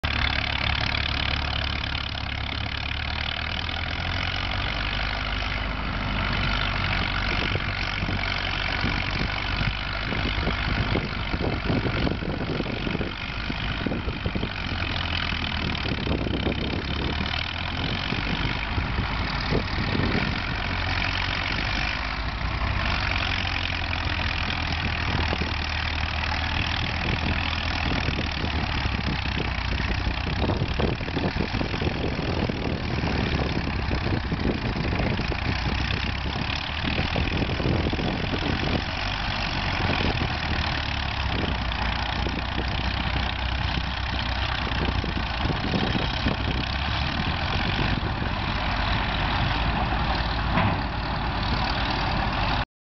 Glen Ghilotti's Playday with Chapter 5 ACMOC moving dirt days '09, #12
Glen Ghilotti's  Caterpillar D4-7u & 40 scraper